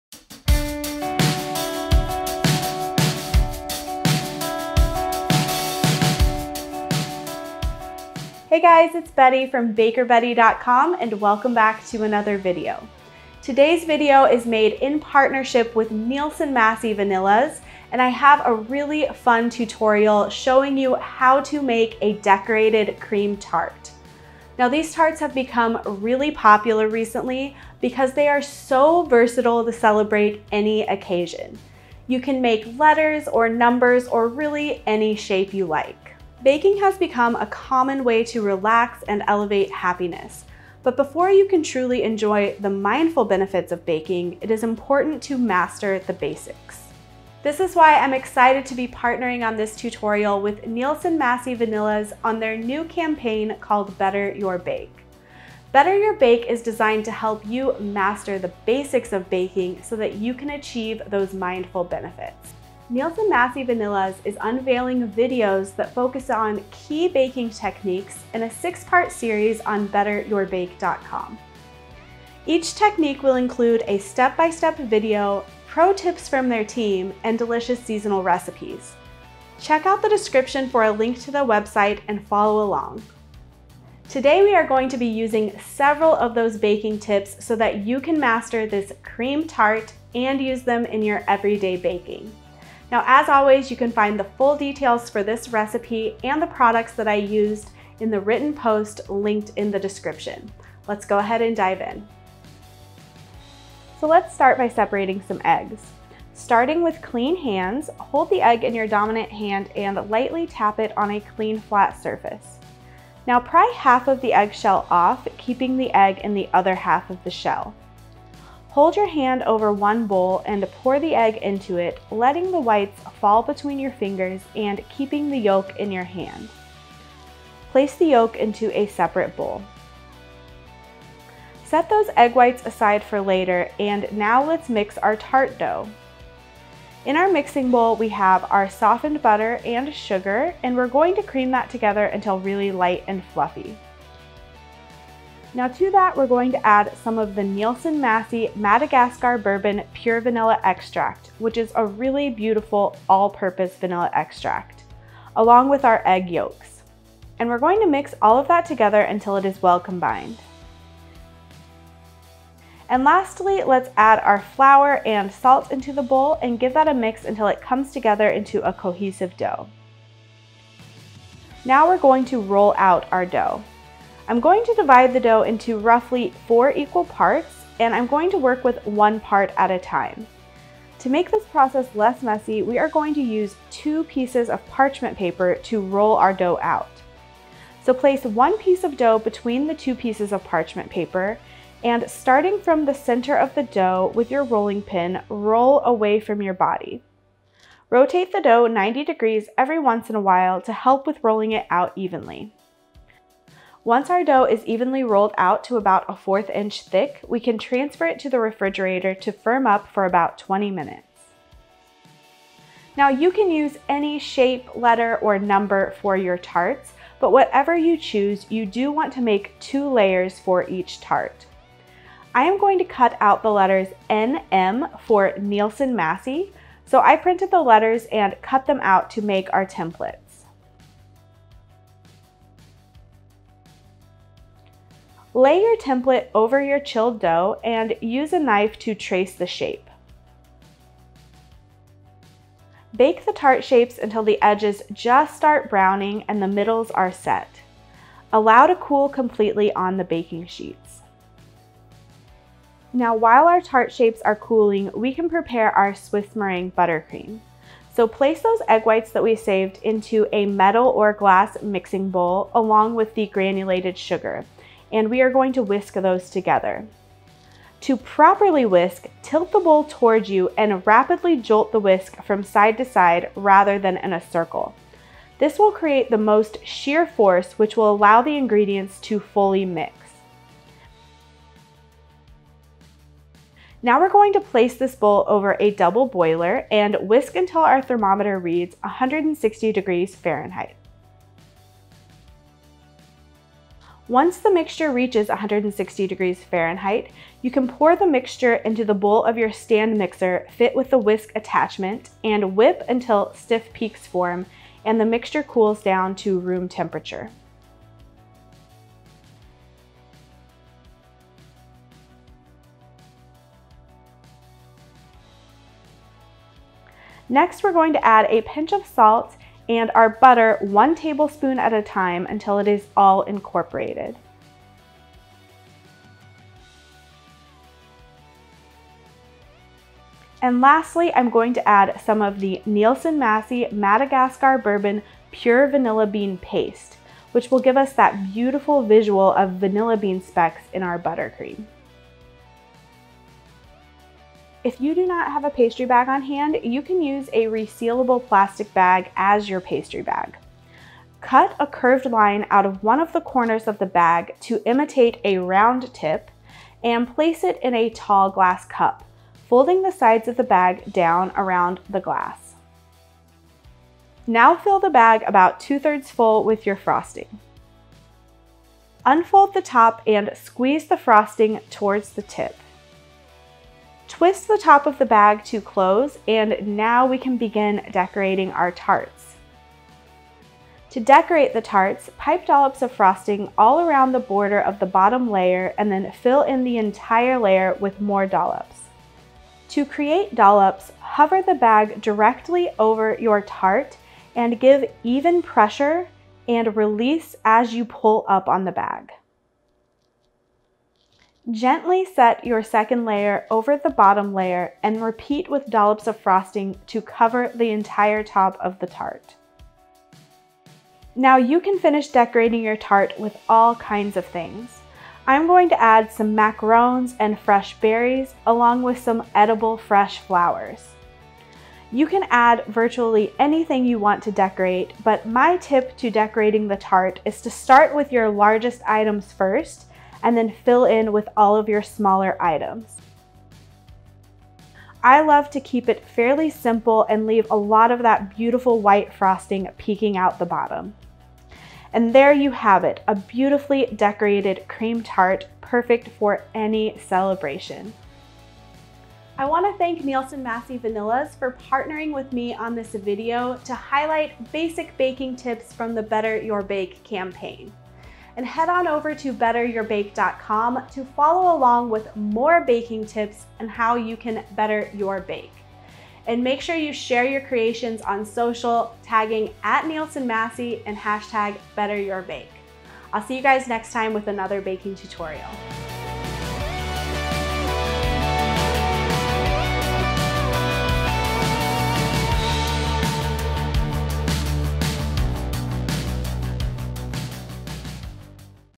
How to Make a Cream Tart Cake | letter tart, alphabet tart
Hand Mixer
Rolling Pin
Sheet Pan
Stand Mixer
Silicone Spatula
Nielsen-Massey Products

INGREDIENTS:
For the Cookie Dough:
2 sticks (8 oz, 220 gr) unsalted butter, room temperature
½ cup (3.5 oz, 100 gr) granulated sugar
3 large egg yolks
1 ½ tsp Nielsen-Massey Madagascar Bourbon Pure Vanilla Extract
¼ tsp kosher salt
2 ¾ cups (11.8 oz, 330 gr) all-purpose flour

For the Swiss Meringue Buttercream:
6 large egg whites
2 cups (14 oz, 395 gr) granulated sugar
¼ tsp kosher salt
4 sticks (1 lb, 440 gr) unsalted butter, room temperature
1 TBSP (15 ml) Nielsen-Massey Madagascar Bourbon Pure Vanilla Bean Paste

To Decorate:
Fresh berries: strawberries, raspberries, and blueberries
Edible flowers (*optional)
Macarons (*optional)
A cream tart, also known as a letter tart or alphabet tart, is a beautiful decorated pastry perfect for any occasion. They are frequently cut into letters or numbers but can be made into any shape you like. The tart shell is made from a shortbread cookie and it is filled with a Swiss Meringue buttercream. The cream tart is then decorated with macarons, fresh fruit, edible fresh flowers.